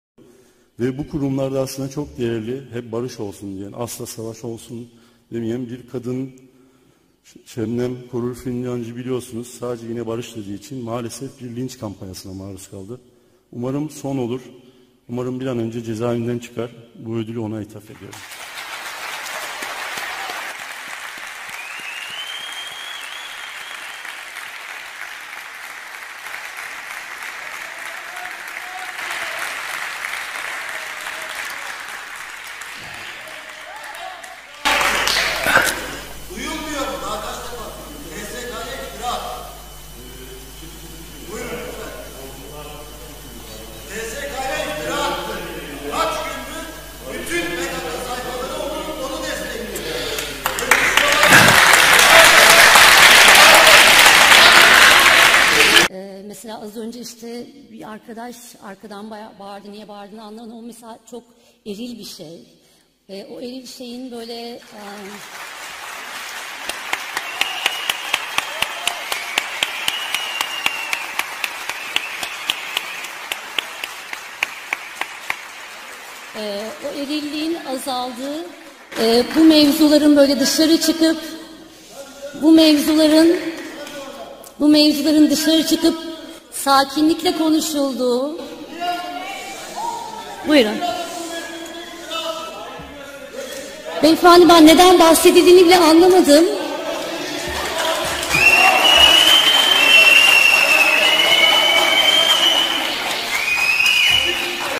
Oyuncu Merve Dizdar'ın tepki çeken 'eril dil' alkışı - Şebnem Korur Fincancı & pkk propagandası
Oyuncu Burak Haktanır, Boğaziçi Film Festivali'nde aldığı ödülü Şebnem Korur Fincancı'ya ithaf eden Özcan Alper'e tepki gösterdi:

👉🏻 “O kadın TSK'ya iftira attı.”

📌 Selcen Ergün, milli duruş gösteren Burak Haktanır'ı "eril dil" kullandığı gerekçesiyle eleştirdi.